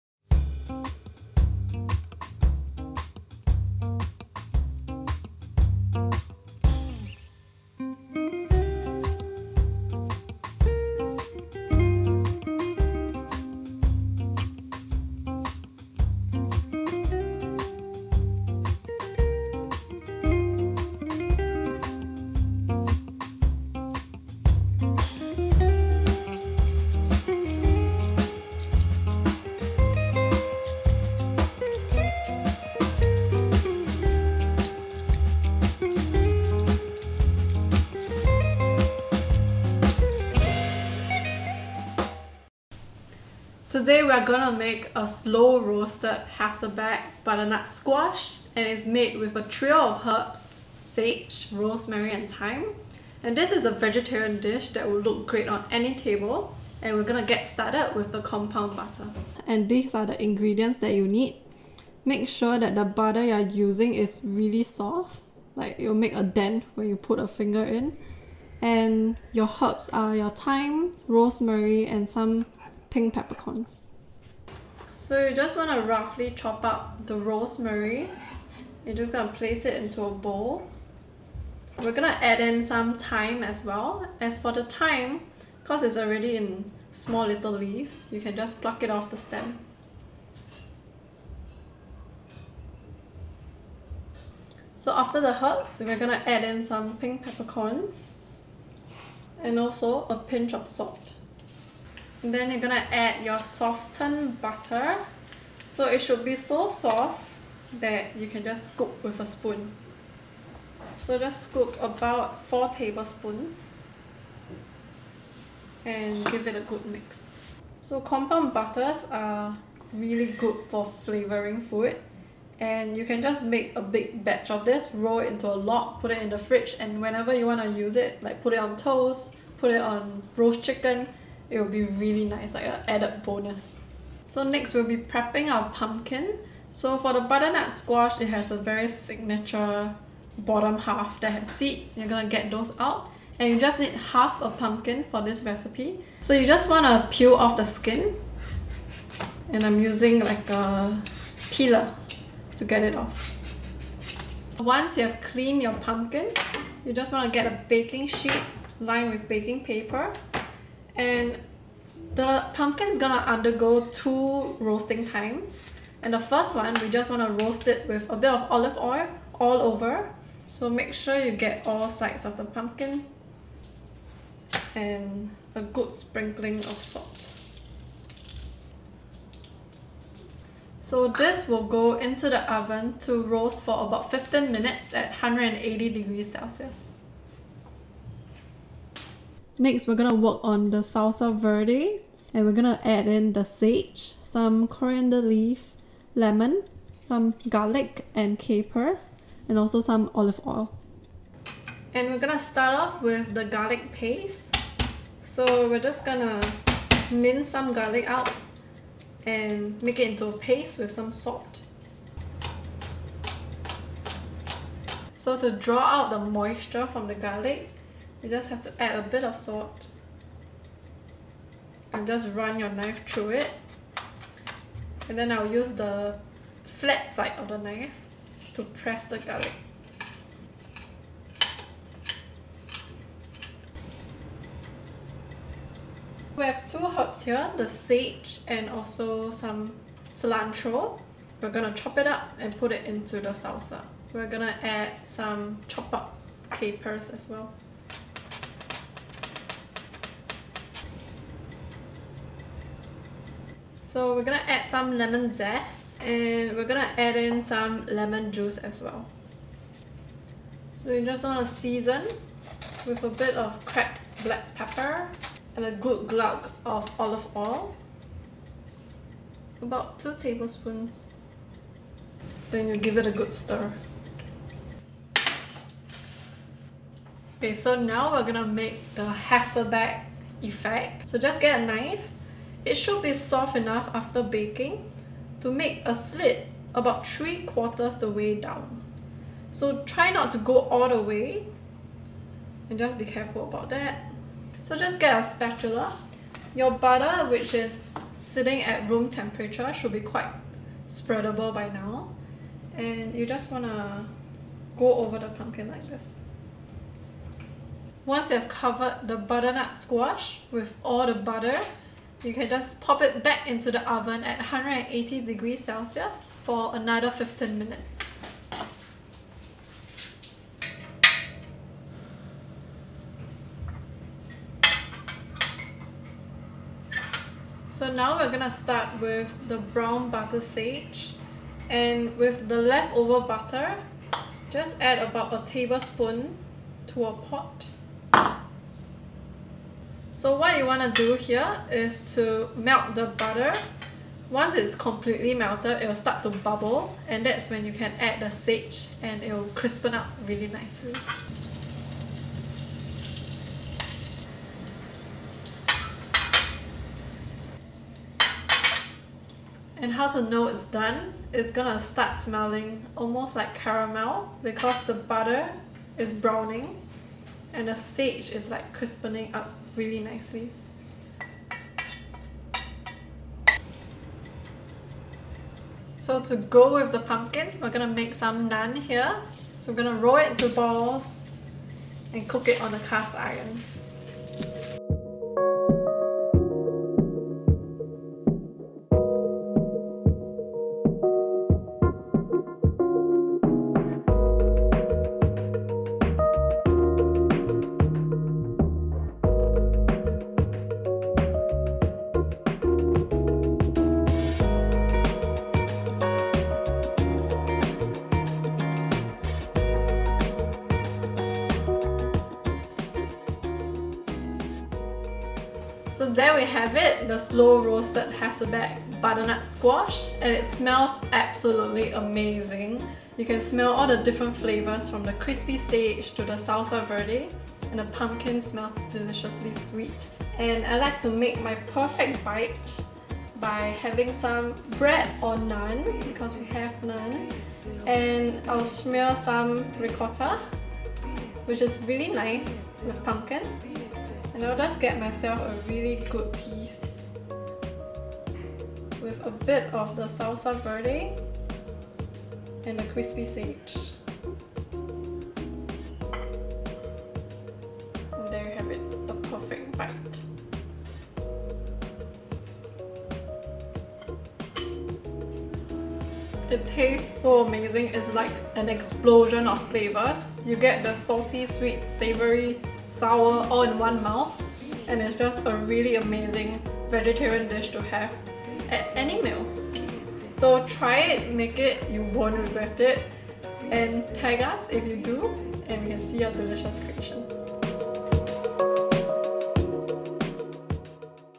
Slow Roasted Hasselback Butternut Pumpkin
There is no better way to fill your home with herby aromatics and have a simple yet delicious dish. This butternut pumpkin recipe is sure to gain praises for your culinary skills!

-  Slow Roasted Hasselback Butternut Pumpkin (serves 4) -

Ingredients:
Half a butternut pumpkin, peeled and cleaned
8 sage leaves
¼ cup Ricotta
1x recipe Naan, 6 pc.

Compound butter:
100g butter, softened
1 sprig of rosemary, chopped
15g of thyme leaves
1 tbsp pink peppercorn
1 tsp salt

Sage salsa verde:
10g sage leaves
20g cilantro leaves
1 tbsp capers
½ lemon zest and juice
2 cloves of garlic, made into a paste
2 tbsp olive oil

Method:
In a bowl, mix all the compound butter ingredients together. Keep it in cling wrap, rolled into a log. This can be kept for 2 weeks.
Line a baking tray with paper and oil your butternut squash all over and season. Bake in the oven for 15mins at 180 degree celsius.
In the meantime, we will work on the Salsa Verde. Chop up the sage, cilantro and capers.
Mix the herbs and capers in the garlic paste, lemon zest, juice, olive oil and seasoning. Set aside.
Take out the butternut squash and make cuts ¾ the way deep into the squash. Spread the butter all over the squash and bake for another 15 mins at 180 degrees celsius.
In a pot, melt 2 tbsp of compound butter. Add the sage leaves and cook till the sage has crisped up, and the butter is browned.
After the squash is done, serve it up with a generous topping of salsa verde and crispy sage.
Tip: Use the leftover browned butter to brush the naan.
Serve the squash with naan schmear'd with ricotta.